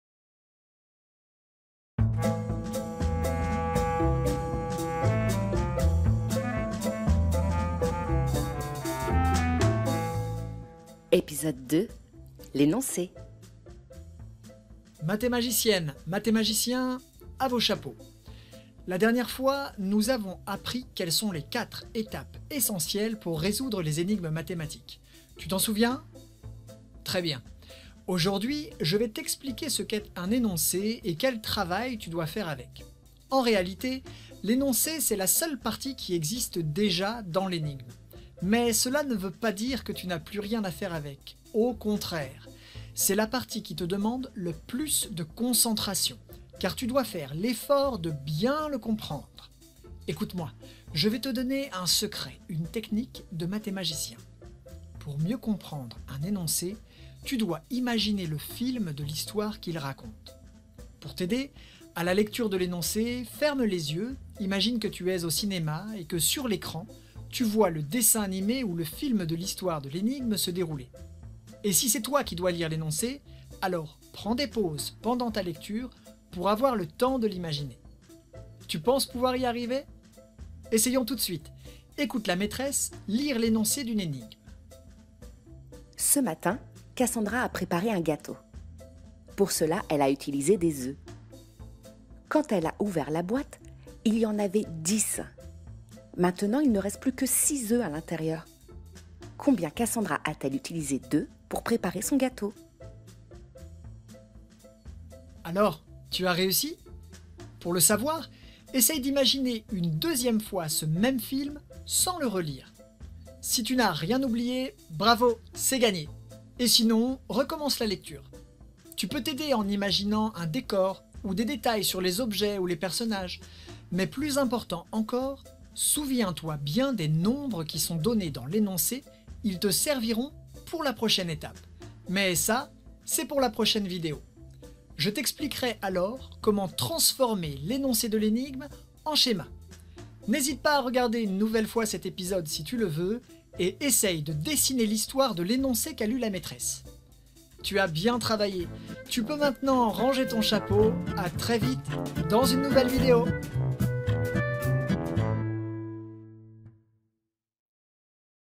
Les Énigmes des Mathémagicien·nes ! 2 - l'énoncé
Mathémagicien·nes, à vos chapeaux !

La dernière fois, nous avons appris quelles sont les 4 étapes essentielles pour résoudre les énigmes mathématiques.
Tu t'en souviens ? Très bien.
Aujourd'hui, je vais t'expliquer ce qu'est un énoncé, et quel travail tu dois faire avec.
En réalité, l'énoncé, c'est la seule partie qui existe déjà dans l'énigme !

Mais, cela ne veut pas dire que tu n'as plus rien à faire avec... au contraire ! C'est la partie qui te demande le plus de concentration car tu dois faire l'effort de bien le comprendre.
Écoute-moi, je vais te donner un secret, une technique de Mathémagicien·ne.

Pour mieux comprendre un énoncé, tu dois imaginer le film de l'histoire qu'il raconte.

Pour t'aider, à la lecture de l'énoncé, ferme les yeux, imagine que tu es au cinéma, et que sur l'écran, tu vois le dessin animé ou le film de l'histoire de l'énigme se dérouler.
Et si c'est toi qui doit lire l'énoncé, alors prends des pauses, pendant ta lecture, pour avoir le temps de l'imaginer..

Tu penses pouvoir y arriver ?
Essayons tout de suite ! Écoute la maîtresse lire l'énoncé d'une énigme...
Ce matin, Cassandra a préparé un gâteau ce matin. Pour cela, elle a utilisé des œufs. Quand elle a ouvert la boîte, il y en avait 10. Maintenant, il ne reste plus que 6 œufs à l'intérieur.
Combien Cassandra a-t-elle utilisé d’œufs pour préparer son gâteau ?

Alors, tu as réussi ?
Pour le savoir, essaye d'imaginer une deuxième fois ce même film, sans le relire. Si tu n'as rien oublié, bravo, c'est gagné ! Et sinon, recommence la lecture !
Tu peux t'aider en imaginant un décor, ou des détails sur les objets ou les personnages, mais plus important encore, souviens-toi bien des nombres qui sont donnés dans l'énoncé, ils te serviront pour la prochaine étape !